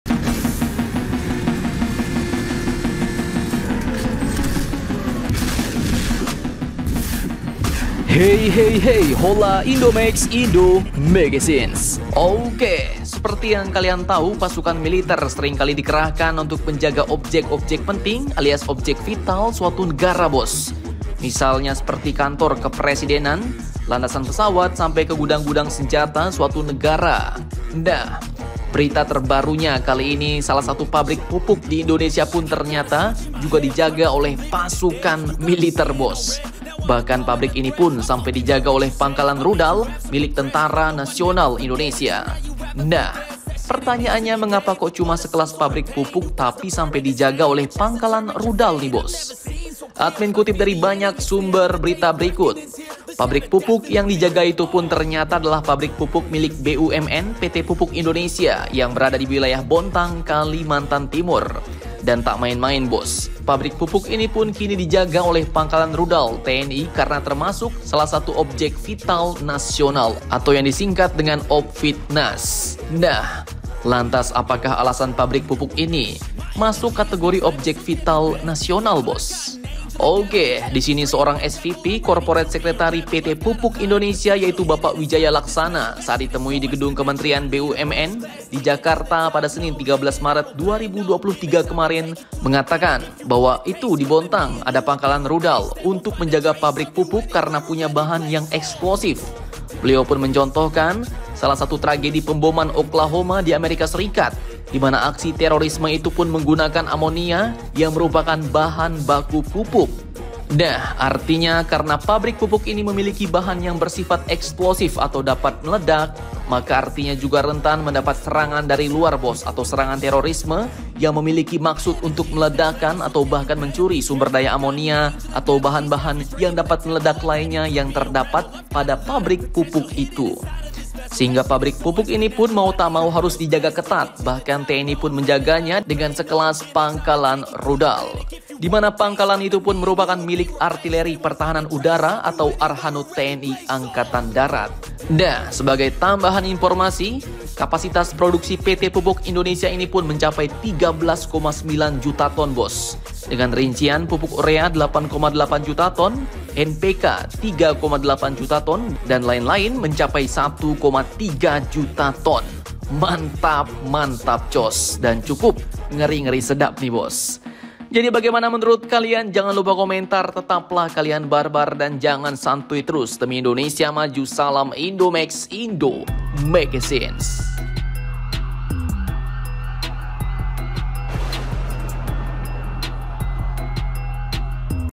NGERI PABRIK PUPUK INDONESIA INI DIJAGA LANGSUNG OLEH PANGKALAN RUDAL TNI, ADA APA ??
PABRIK PUPUK INDONESIA INI DIJAGA LANGSUNG OLEH PANGKALAN RUDAL TNI, ADA APA ??
Okeehh Seperti yang kalian tau pasukan militer sering kali dikerahkan untuk menjaga objek2 penting alias objek vital suatu negara,
Misalnya seperti kantor kepresidenan, landasan pesawat sampai ke gudang2 senjata suatu negara
Nah berita terbaru nya kali ini
Salah satu pabrik pupuk di Indonesia pun ternyata juga dijaga oleh pasukan militer bos,,
Bahkan pabrik ini sampai dijaga oleh pangkalan rudal milik Tentara Nasional Indonesia
Nahhh mengapa kok Cuma sejekas pabrik pupuk tapi sampai dijaga oleh pangkalan rudal nih bos
Admin kutip dari sumber berita berikut
Pabrik pupuk yang dijaga itu ternyata adalah pabrik pupuk miliki BUMN PT Pupuk Indonesia yang berada di wilayah Bontang, Kalimantan Timur
Dan tak main main bos pabrik pupuk ini pun kini dijaga oleh pangkalan rudal TNI karena termasuk salah satu objek vital nasional.
Nah lantas apa alasan pabrik pupuk ini masuk kategori objek vita nasional
Seorang SVP Corporate Secretary PT Pupuk Indonesia yaitu bapak Wijaya Laksana saat ditemui di gedung Kementerian BUMN, di Jakarta pada Senin 13 Maret 2023.  Kemarin
Mengatakan bahwa Itu di Bontang ada pangkalan rudal untuk menjaga pabrik pupuk karena punya bahan yang eksplosif